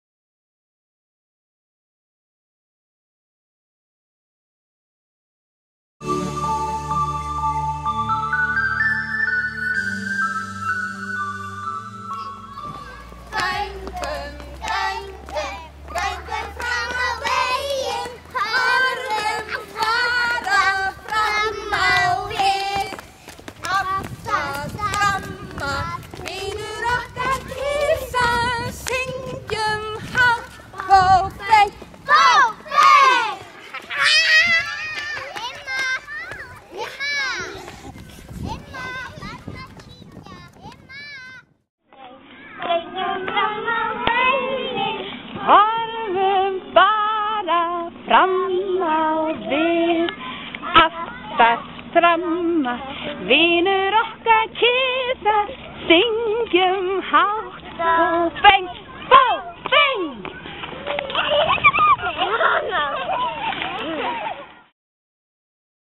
Mabela the Clever
This is a game suggested by Margaret Read MacDonald in her book "Mabela the Clever". The book is about a little mouse that sees through the evil plan of the cat. The cat is marching behind all the mice and one by one (when they chant "FO Feng!") it catches them and puts them in a bag. Mabela is in the front and suddenly she realizes what is going on due to the good advice her father gave her: listen, look, pay attention and run...